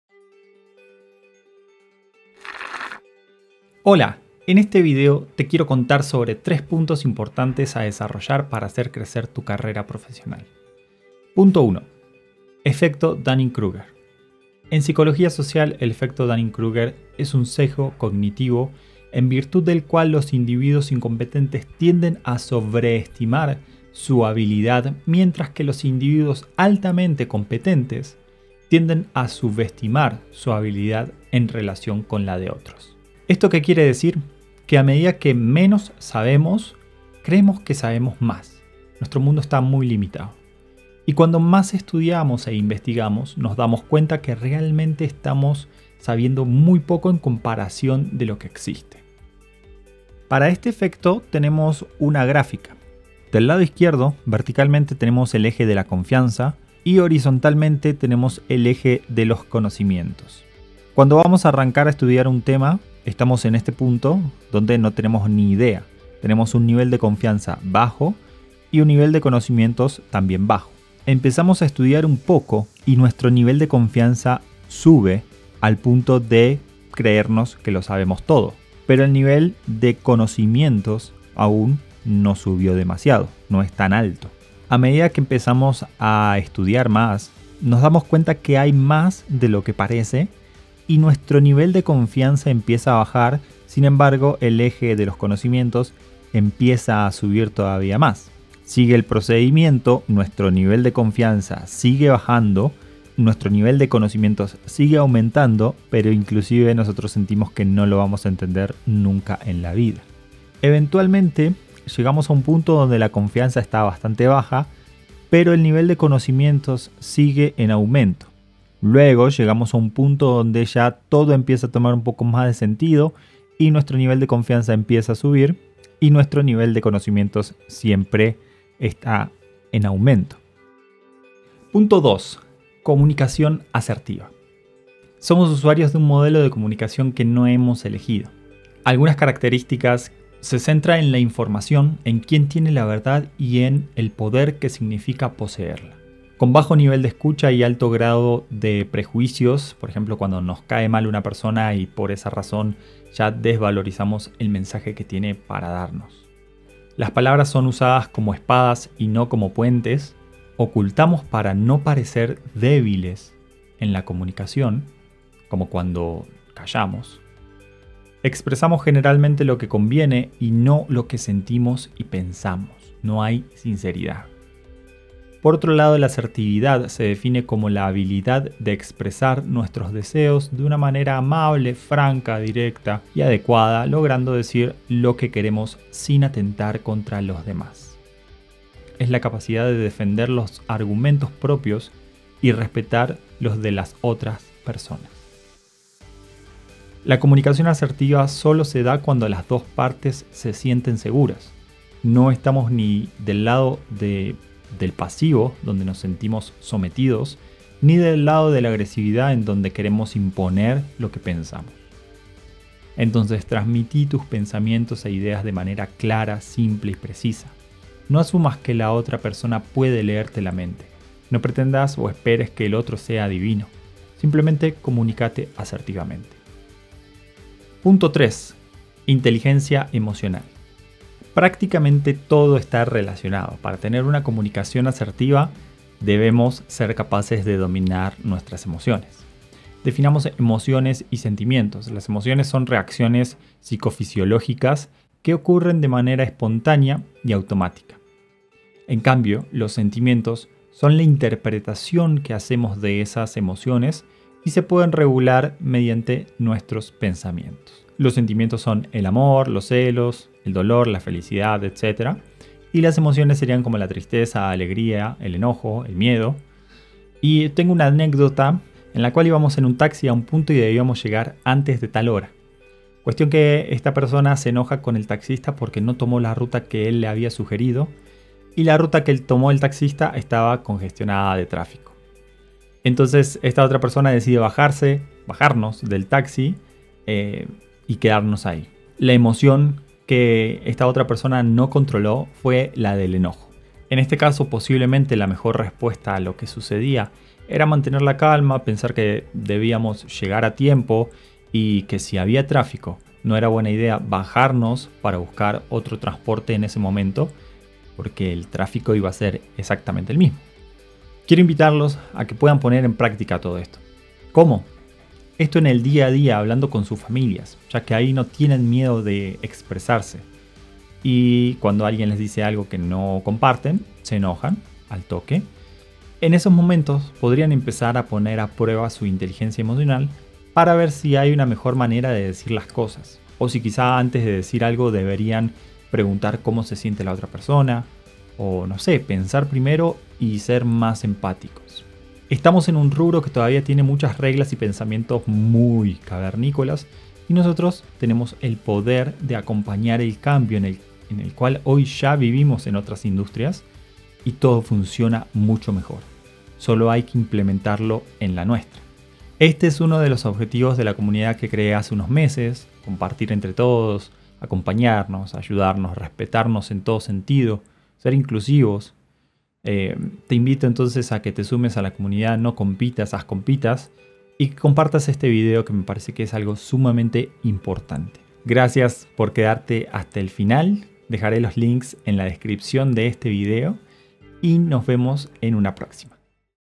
3 habilidades blandas que debes fortalecer para una mejor carrera profesional!
Existe un efecto llamado Dunning-Kruger que dice que los individuos con escasa habilidad o conocimientos sufren de un sentimiento de superioridad ilusorio, considerándose más inteligentes que otras personas más preparadas, midiendo incorrectamente su habilidad por encima de lo real.
Veamos esto y dos puntos más en este video!